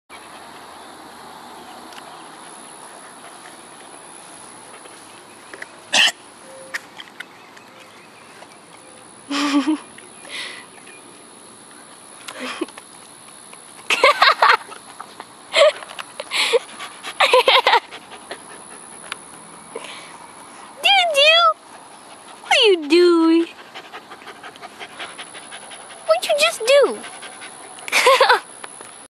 What is my dog doing???????
My dog made this weird face and started to lick her lips and make weird sounds. What in the world is she doing?